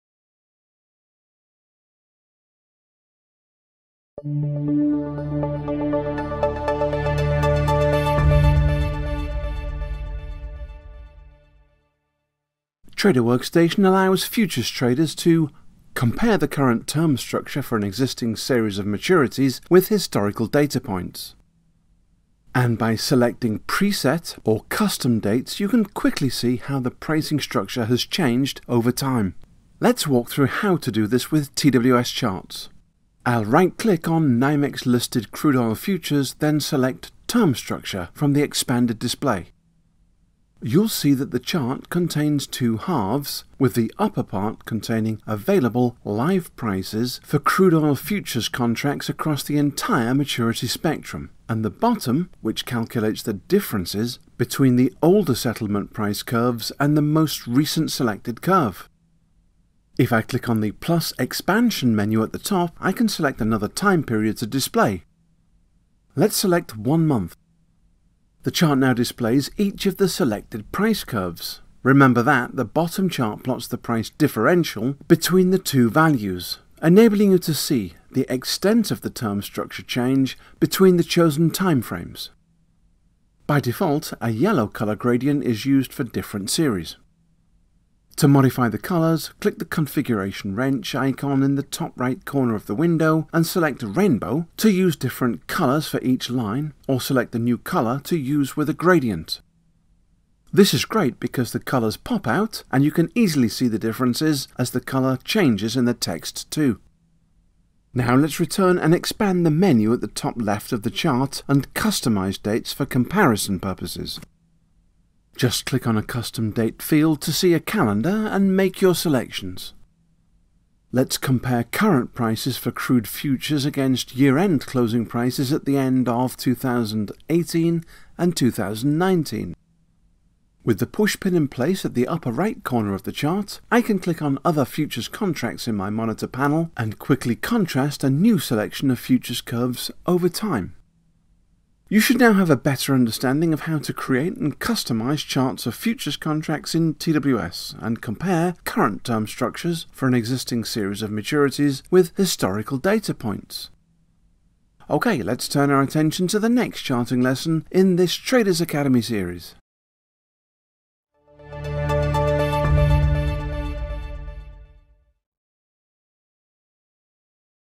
TWS Charts Futures Term Structure
Learn how to view future term structures in TWS Charts which enable you to:
• Compare the current term structure for an existing series of maturities with historical data points.
• Customize the term by selecting preset or custom dates, you can quickly see how the pricing structure has changed over time.